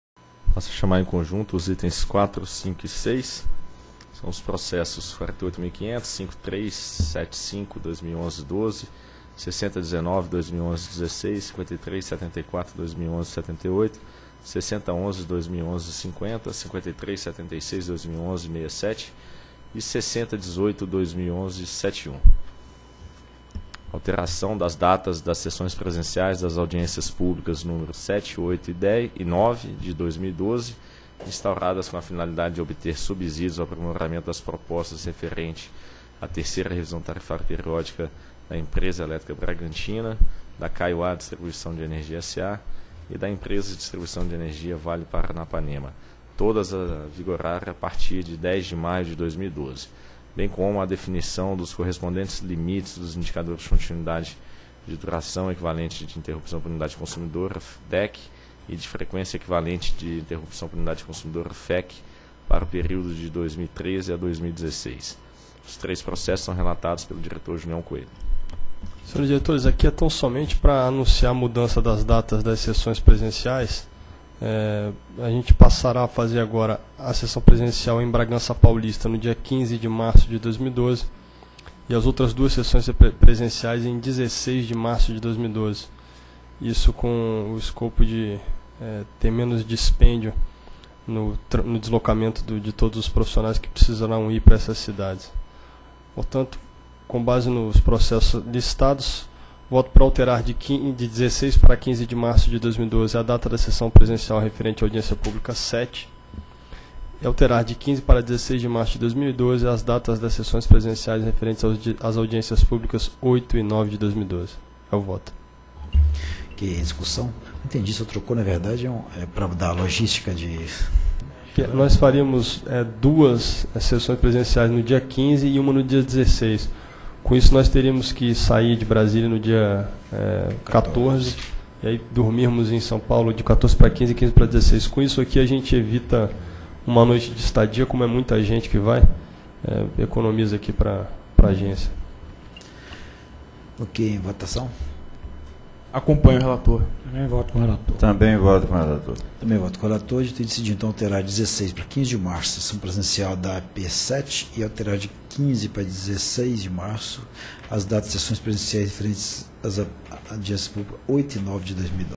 Item 4 ao Item 6 - 7ª Reunião Pública Ordinária ANEEL 06/03/2012
Item 4. Processo nº: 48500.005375/2011-12 e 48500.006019/2011-16. Assunto: Alteração da data da sessão presencial da Audiência Pública nº 7/2011, instaurada com vistas a colher subsídios para o aprimoramento da proposta referente à Terceira Revisão Tarifária periódica da Empresa Elétrica Bragantina -- EEB, a vigorar a partir de 10 de maio de 2012; e para a definição dos correspondentes limites dos indicadores de continuidade de Duração Equivalente de Interrupção por Unidade Consumidora -- DEC e de Frequência Equivalente de Interrupção por Unidade Consumidora -- FEC, para o período de 2013 a 2016. Área Responsável: Superintendência de Regulação Econômica -- SRE e Superintendência de Regulação dos Serviços de Distribuição -- SRD.

Item 5. Processo nº: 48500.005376/2011-67 e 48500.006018/2011-71. Assunto: Alteração da data da sessão presencial da Audiência Pública nº 8/2011, instaurada com vistas a colher subsídios para o aprimoramento da proposta referente à Terceira Revisão Tarifária periódica da Empresa de Distribuição de Energia Vale Paranapanema S.A. -- EDEVP, a vigorar a partir de 10 de maio de 2012; e para a definição dos correspondentes limites dos indicadores de continuidade de Duração Equivalente de Interrupção por Unidade Consumidora -- DEC e de Frequência Equivalente de Interrupção por Unidade Consumidora -- FEC, para o período de 2013 a 2016. Área Responsável: Superintendência de Regulação Econômica -- SRE e Superintendência de Regulação dos Serviços de Distribuição -- SRD.

Item 6. Processo nº: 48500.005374/2011-78 e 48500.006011/2011-50. Assunto: Alteração da data da sessão presencial da Audiência Pública nº 9/2011, instaurada com vistas a colher subsídios para o aprimoramento da proposta referente à Terceira Revisão Tarifária periódica da Caiuá Distribuição de Energia S.A. -- CAIUÁ-D, a vigorar a partir de 10 de maio de 2012; e para a definição dos correspondentes limites dos indicadores de continuidade de Duração Equivalente de Interrupção por Unidade Consumidora -- DEC e de Frequência Equivalente de Interrupção por Unidade Consumidora -- FEC, para o período de 2013 a 2016. Área Responsável: Superintendência de Regulação Econômica -- SRE e Superintendência de Regulação dos Serviços de Distribuição -- SRD.